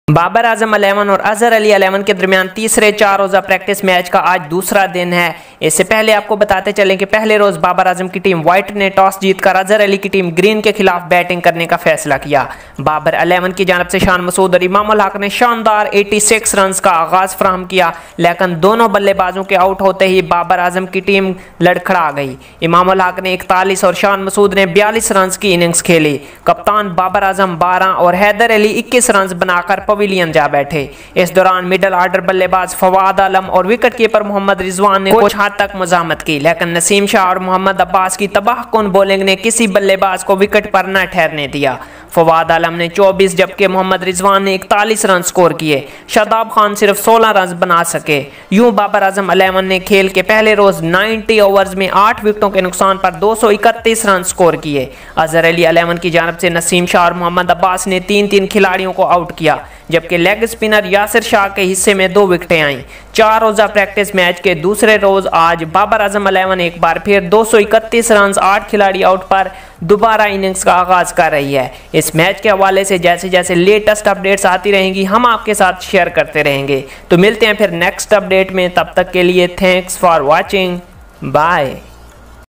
Babar X1 vs Azhar X1 Practice match day 2 Live | Pak cricket team 3rd Practice match 2020 .
About video
In this video we talk about Pak cricket team 4 day Intra Practice match 2020 day 2 Live updates .
Cover Topic
Pak team 3rd intra Practice match 2020
Azhar 11 vs Babar 11 Practice match live updates
Pak vs Eng series 2020